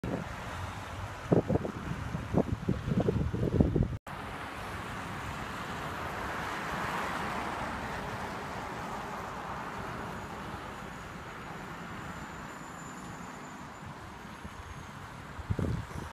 Голый мужчина на остановке в Красноярске
Красноярцы в 20-градусный мороз сняли на видео голого мужчину внутри теплой остановки "Студгородок". Как он там оказался, и почему был раздет - история умалчивает :З